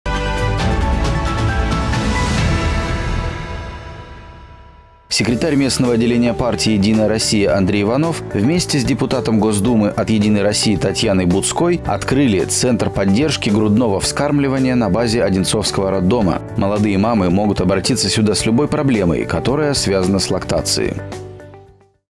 Центр поддержки ГВ
Секретарь местного отделения партии «Единая Россия» Андрей Иванов вместе с депутатом Госдумы от «Единой России» Татьяной Буцкой открыли Центр поддержки грудного вскармливания на базе Одинцовского роддома. Молодые мамы могут обратиться сюда с любой проблемой, которая связана с  лактацией.